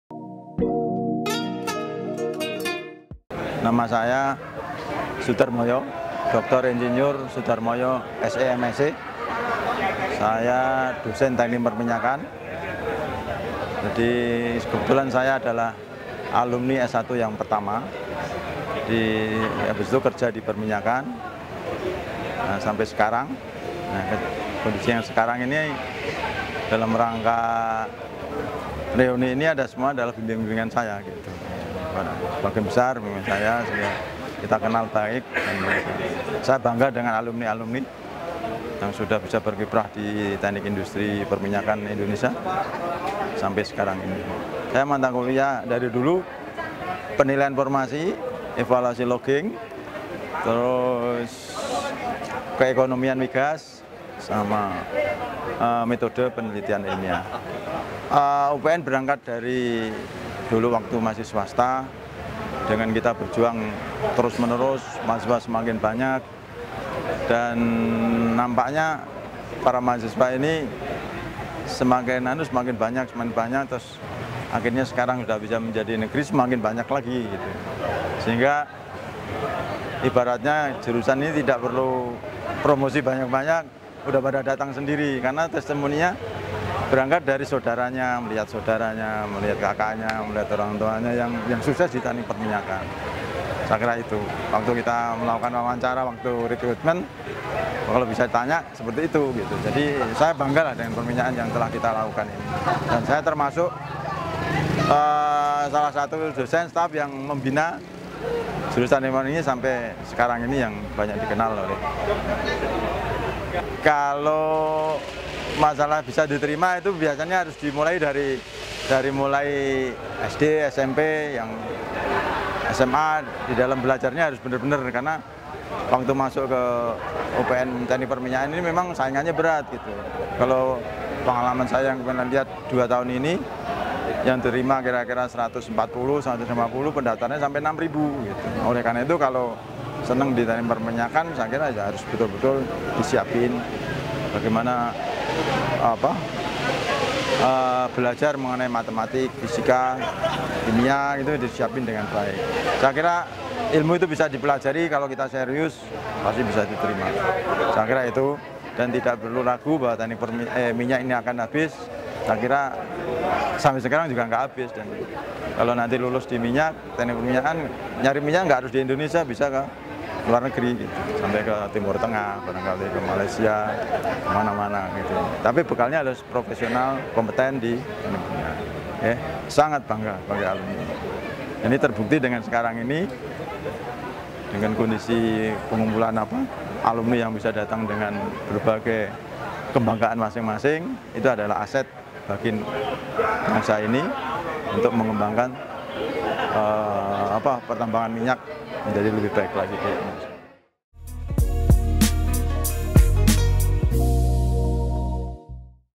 Bangga Dengan Perminyakan Selama Ini - Sudarmoyo
"Jika ingin masuk perminyak harus bener-bener disiapkan karna sainganya ribuan orang" - Sudarmoyo, Tek.Perminyakan 70'